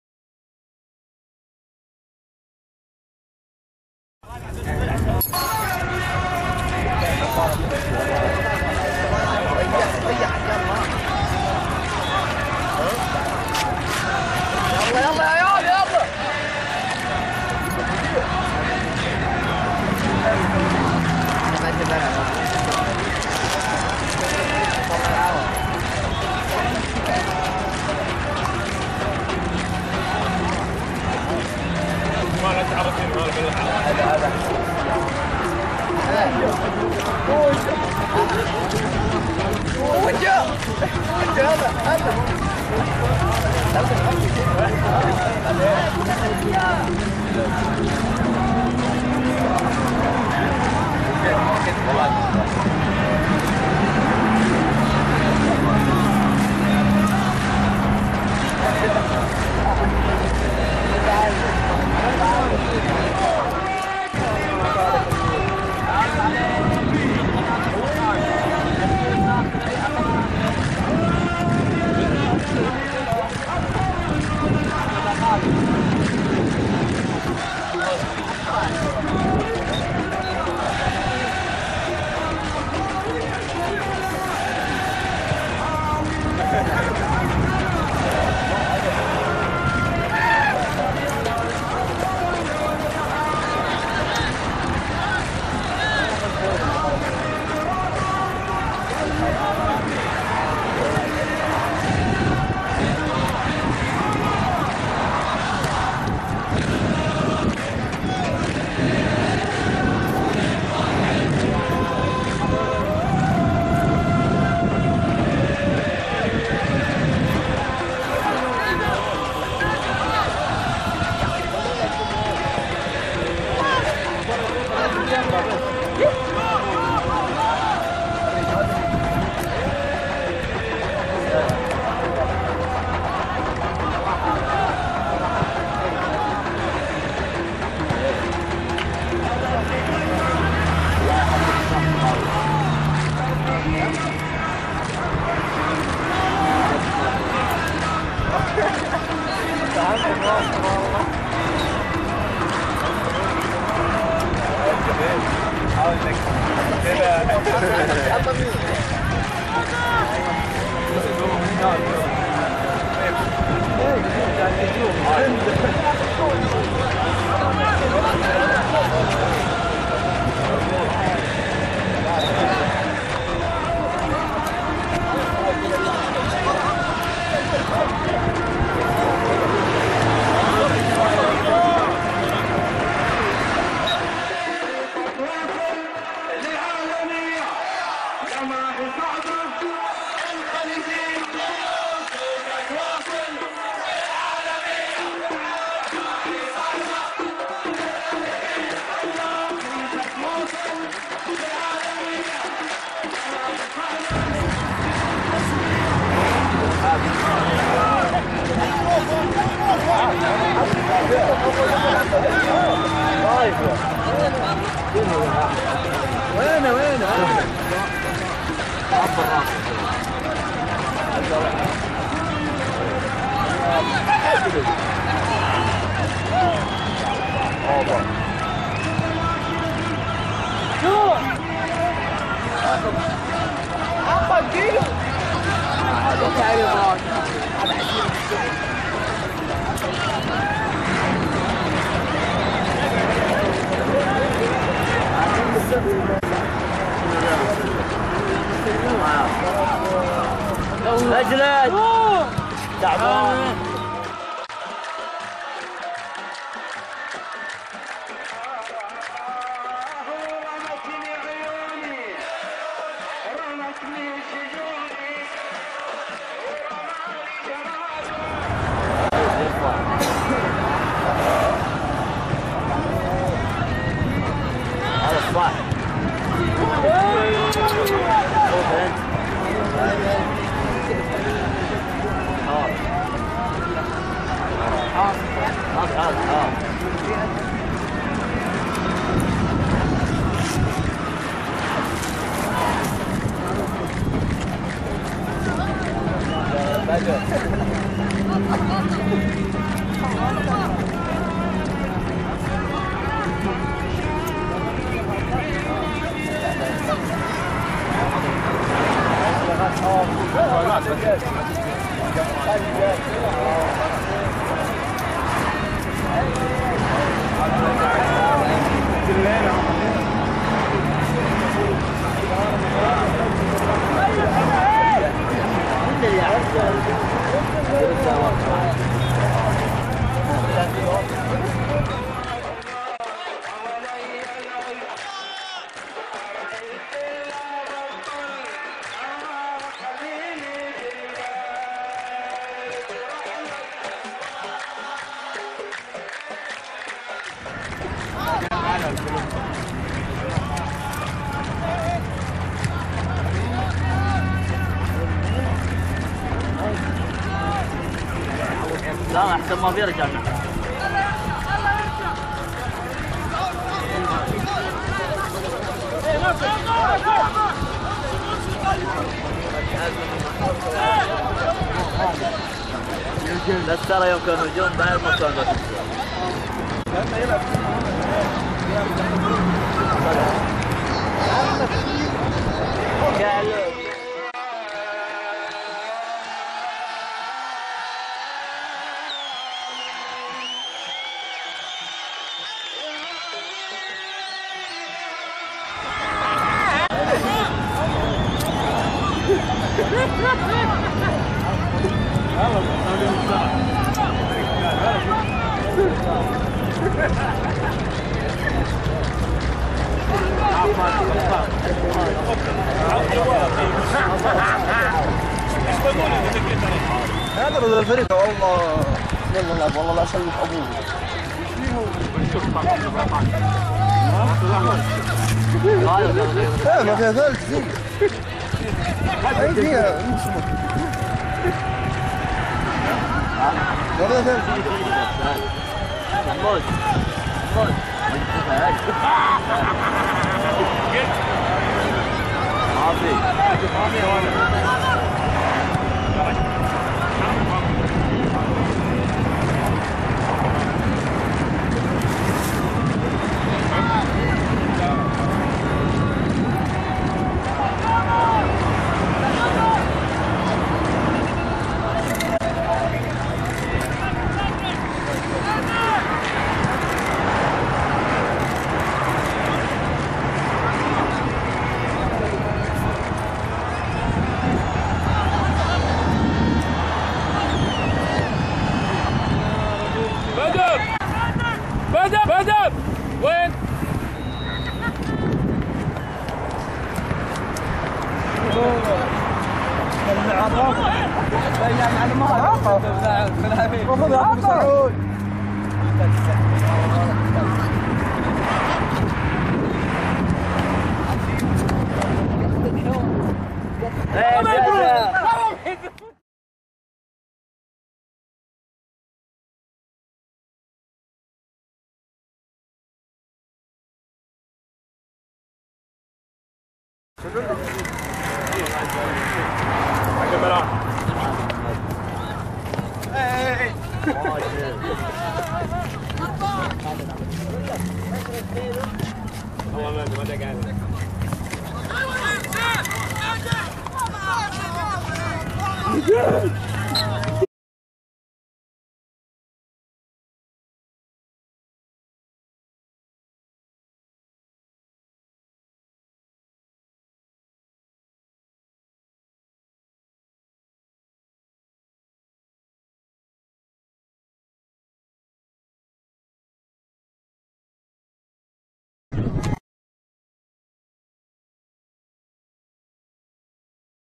ملخص مباراة : الفارس العالمي & التانغو مباراة ودية الشوط الاول 2011
المباراة الاستعدادية السادسة
الخصم فريق التانغو من الدمام